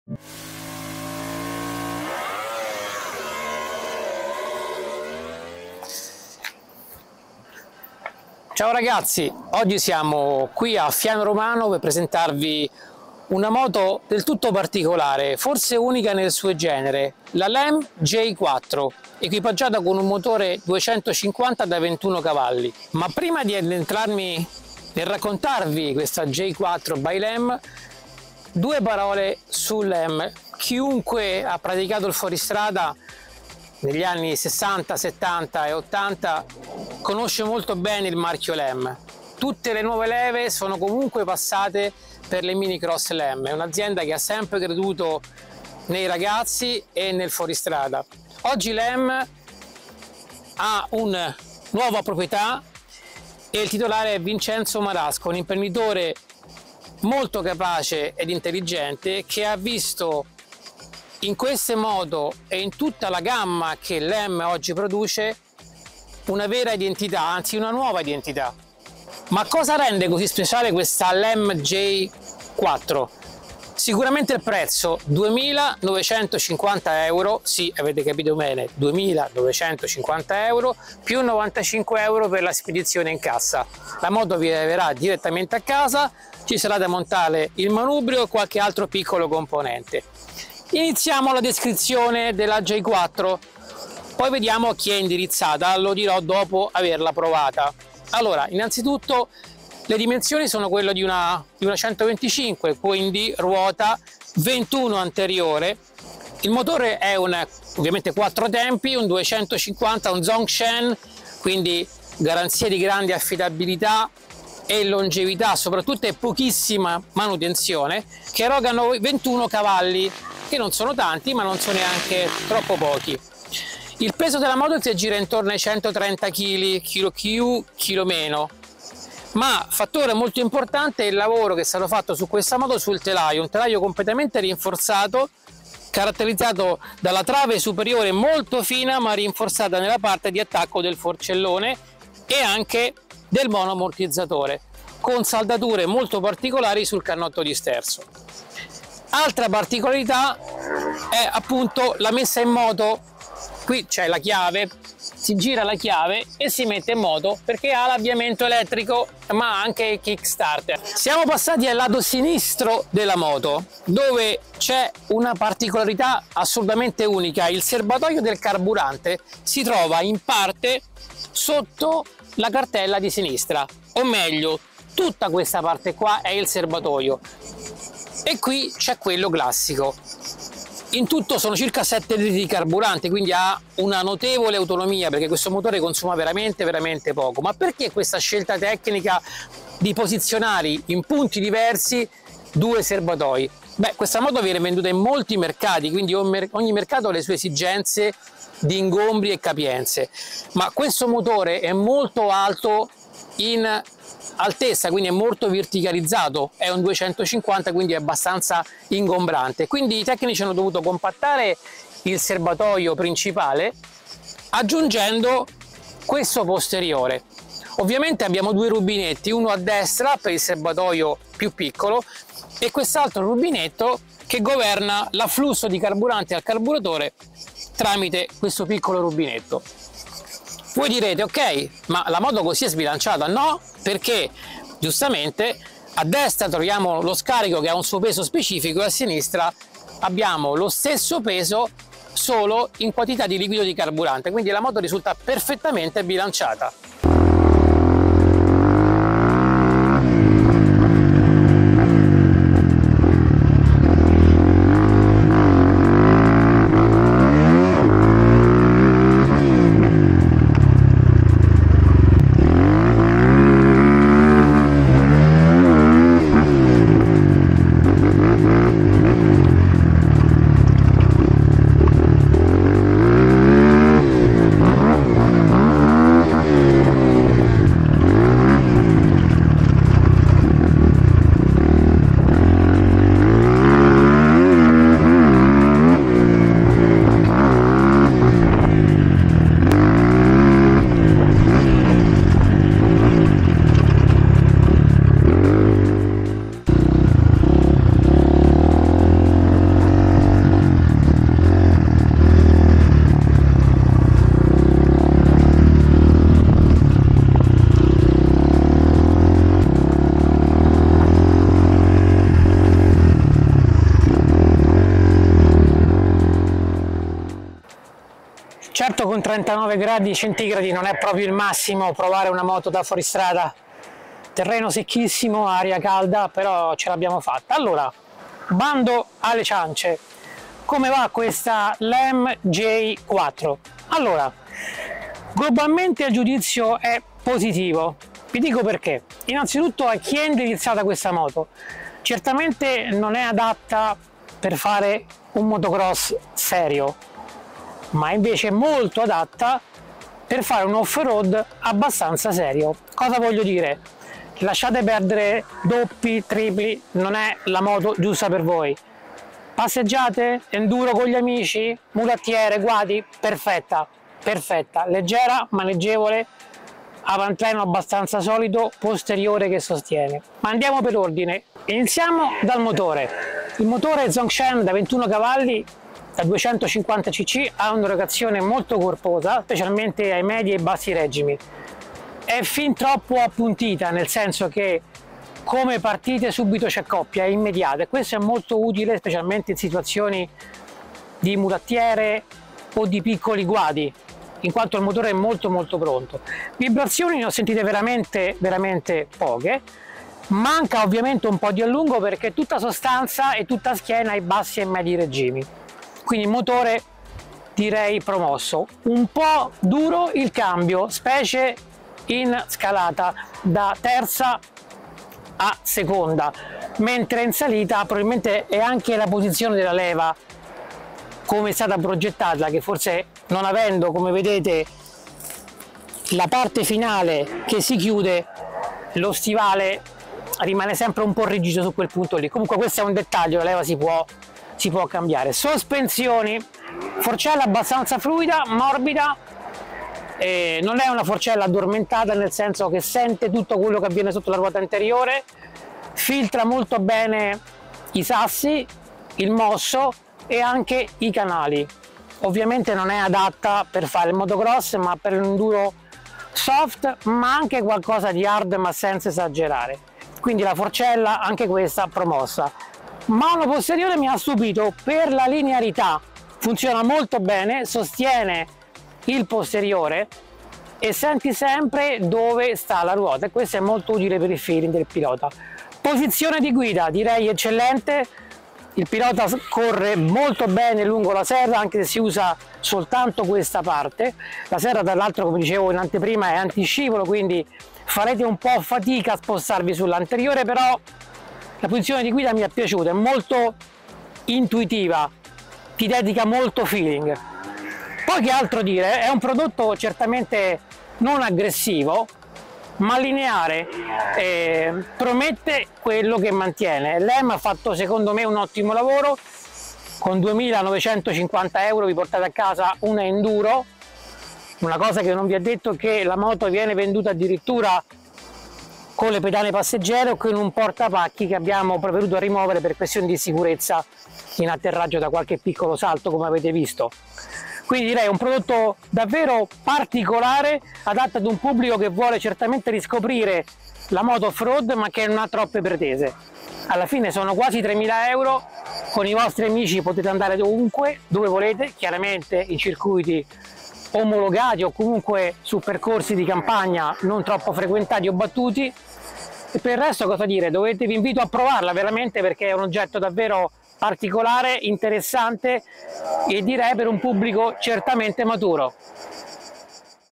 performancemag it Test LEM Dirt Bike J4, la cross/enduro low cost
performancemag it Test LEM Dirt Bike J4, la cross/enduro low cost. La J4 LEM ha tutte le carte in regola per appassionare un pubblico molto vasto di utenza ad un prezzo davvero eccezionale. LEM da anni ha formato nuove leve di pilotini in erba ed oggi propone una moto adatta anche al pubblico adulto o restarter